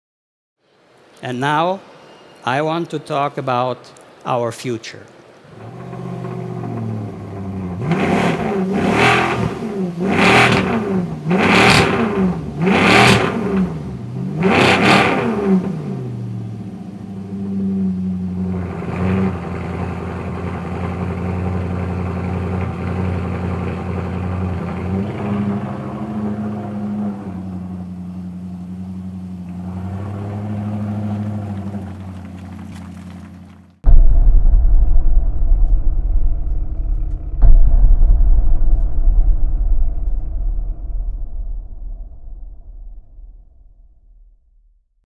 Maserati Alfieri 2+2 Concept unveiling at 2014 Geneva Motor Show
Maserati has introduced in Geneva the stunning Alfieri 2+2 concept which hints at a new production model.